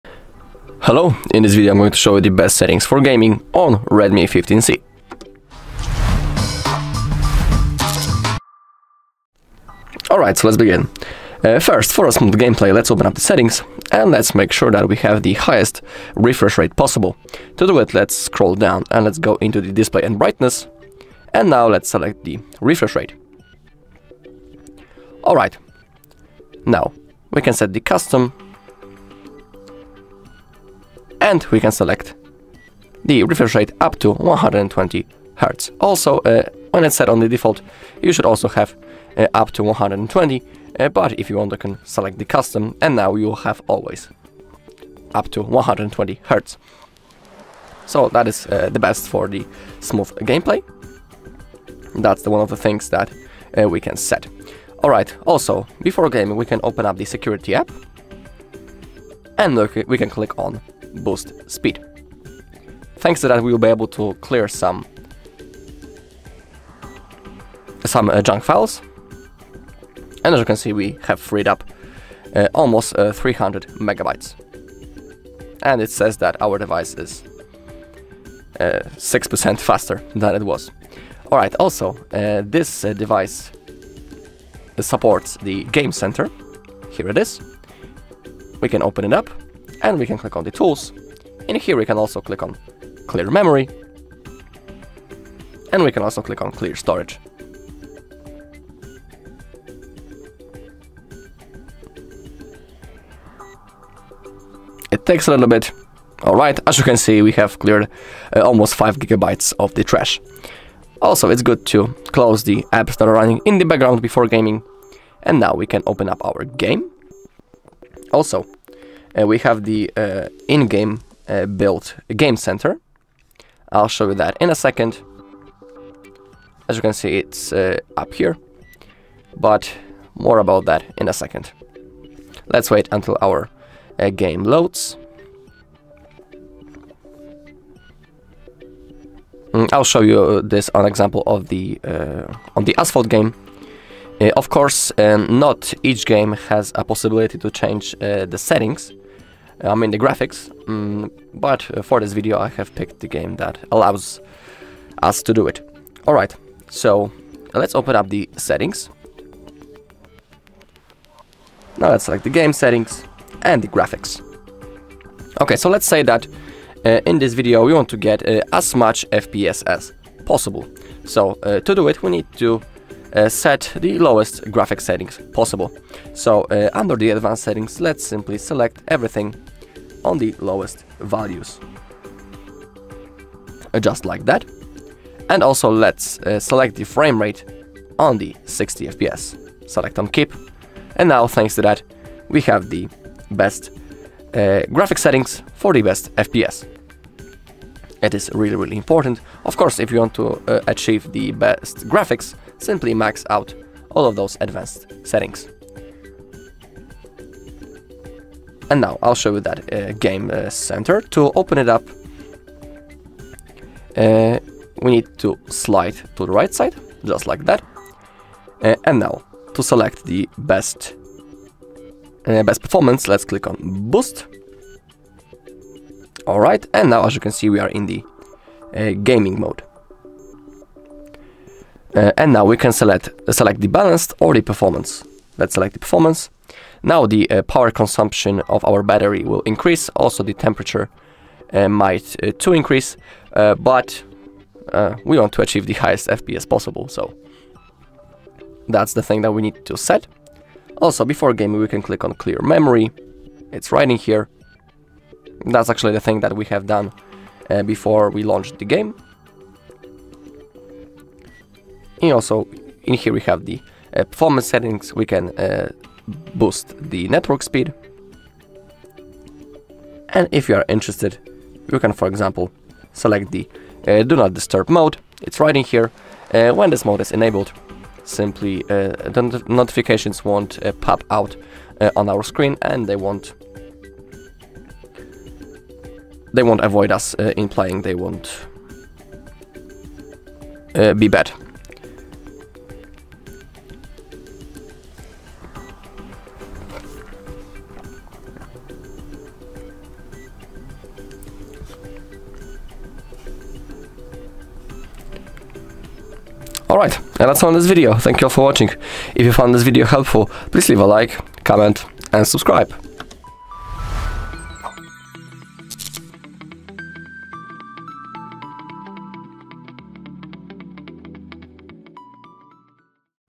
REDMI 15C 4G Best Gaming Settings – How to Optimize Performance
Looking to get the best gaming experience on your REDMI 15C 4G? In this video, we show you the top settings and tips to boost gaming performance, increase FPS, and enjoy smoother gameplay on your REDMI 15C 4G smartphone. Learn how to set the highest refresh rate, clear memory, use Game Center features, adjust graphics for maximum FPS, and enable performance mode. Whether you play Asphalt or other popular games, these steps will help you get the most out of your device for gaming. Follow along to optimize your REDMI 15C 4G for the best possible gaming experience.

How to optimize gaming performance on REDMI 15C 4G?
How to set the best graphics settings for games on REDMI 15C 4G?
How to use Game Center and boost mode on REDMI 15C 4G for gaming?

0:00 Introduction and overview
0:11 Set highest refresh rate (120Hz)
0:54 Clear memory and boost speed
1:20 Use Game Center tools
1:57 Adjust in-game graphics settings
2:34 Enable performance mode in Game Center
3:22 Additional tips (network boost, do not disturb)
4:08 Conclusion